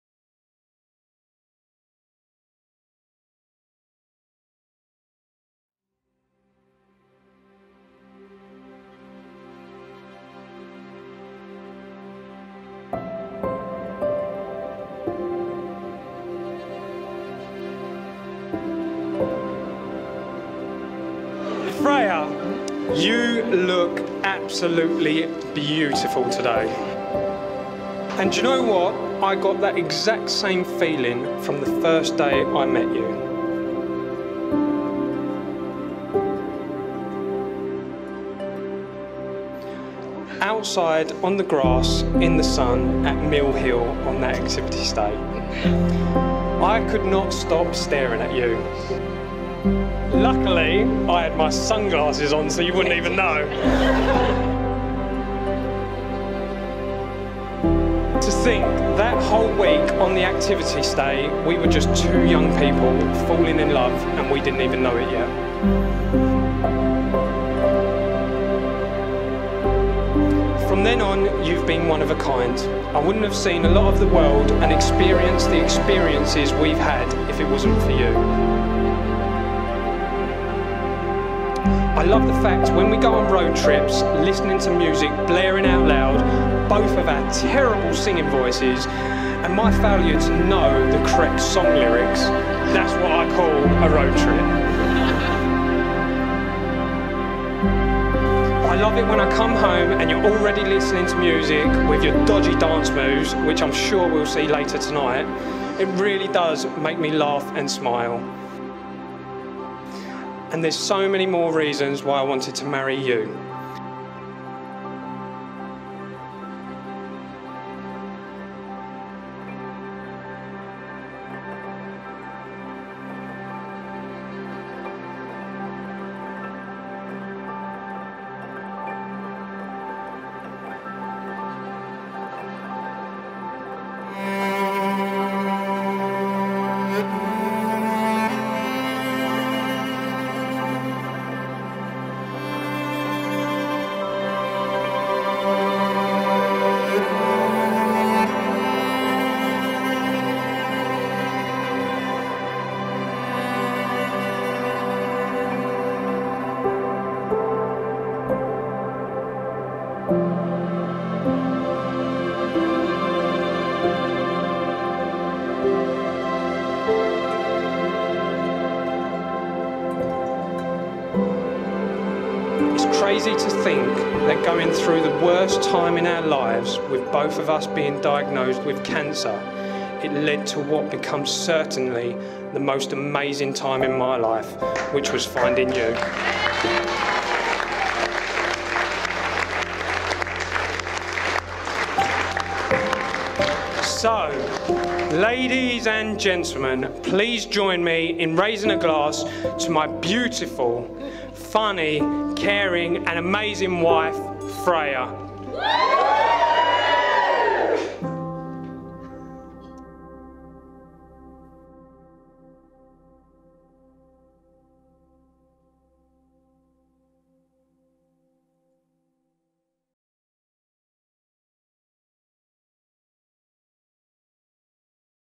Cooling Castle Barn - Freya & Tyler
Frey & Tyler wedding video filmed at Cooling Castle Barn.
Wedding videography by Royal Wedding Films.